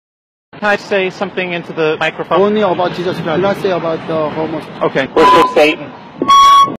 worship satan
ok guys

characters belong to me, i hope you like them. mallory isnt against gays nor is he a christian, but compared to sweetheart, anyone's a saint.
mallory is well-meaning and kind though gets tired of sweetheart's constant abuse.
Also this kind of interaction is impossible, since Sweetheart is a virus inside of Mallory. Sweetheart /is/ Mallory. Just wanted to do this for fun.